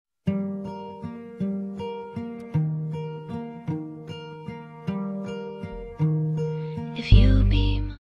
Testing, testing...
I'm just testing out a new effect. It pretty much looks horrible. Haaaa. I'm so bad with effects. Ugh. This will be a complete video at some point. Once I finish the mini-series. :) I've only just watched the first part and I kind of ship these two already.